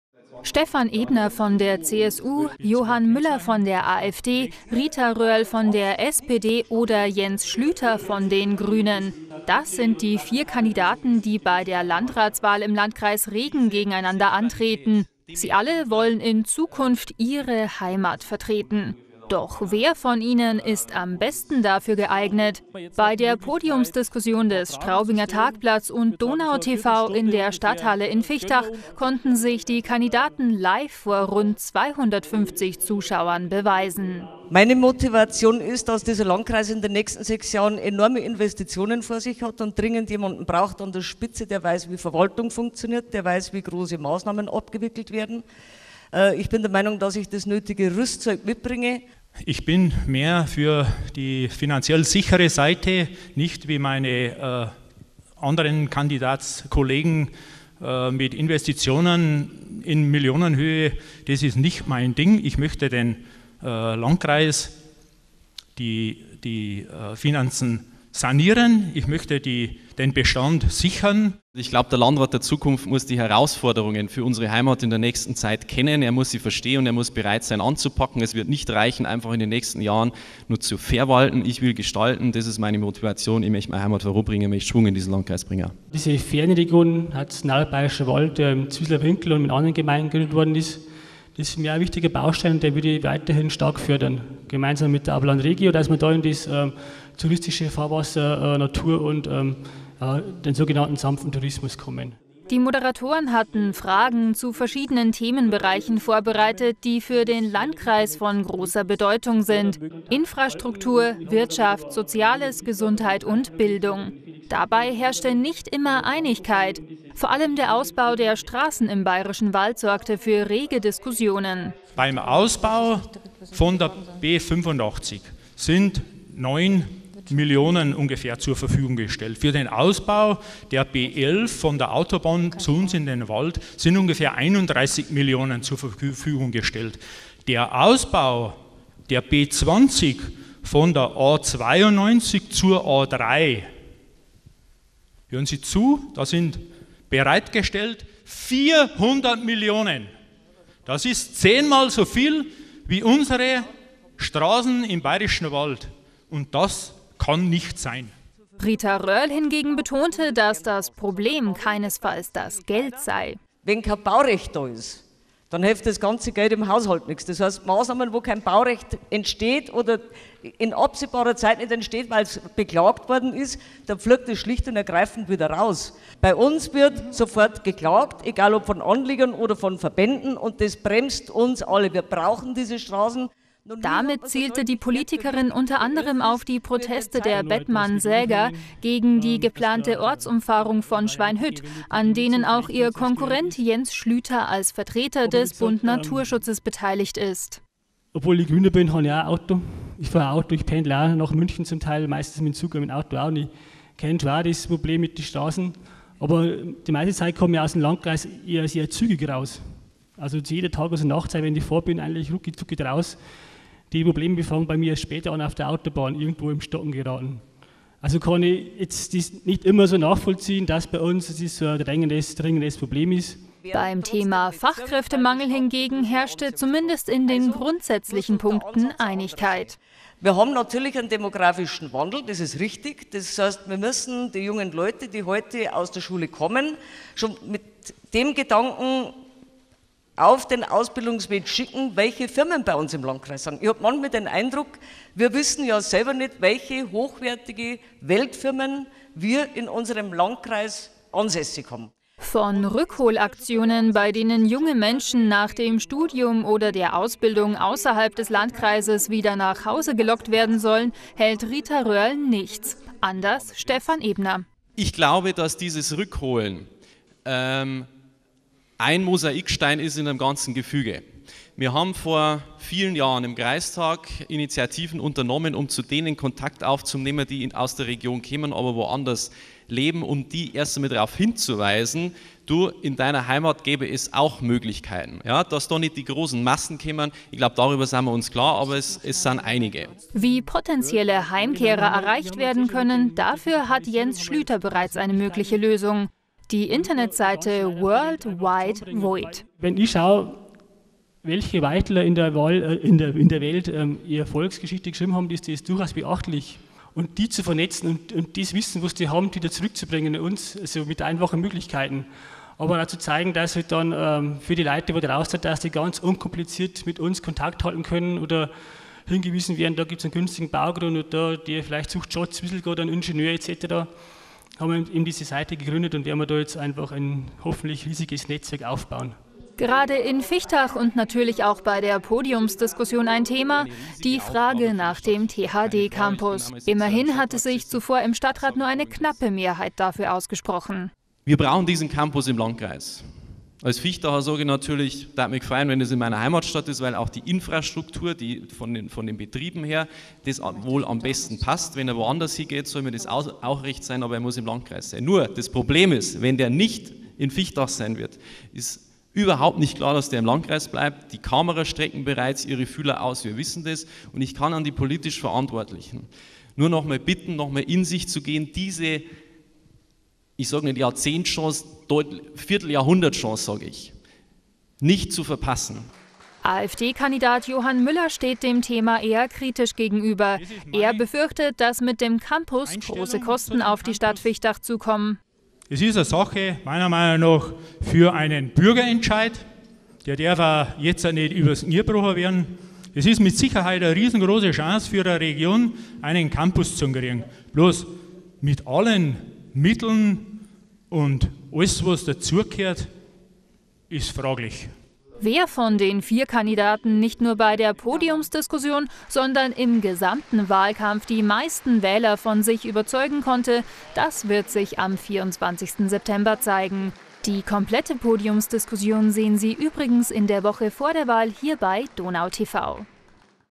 Podiumsdiskussion – mit den Regener Landratskandidaten im Gespräch
Sei es der Ausbau der B11, der Campus in Viechtach oder der Kampf gegen den Demographischen Wandel – knapp einen Monat vor der Landratswahl im Landkreis Regen halten zahlreiche heiß diskutierte Themen die Region auf Trab… ideale Munition für die vier Kandidaten. Wer seine Waffen am besten einsetzen kann, das zeigte sich am Freitagabend bei unserer Podiumsdiskussion in der Stadthalle Viechtach.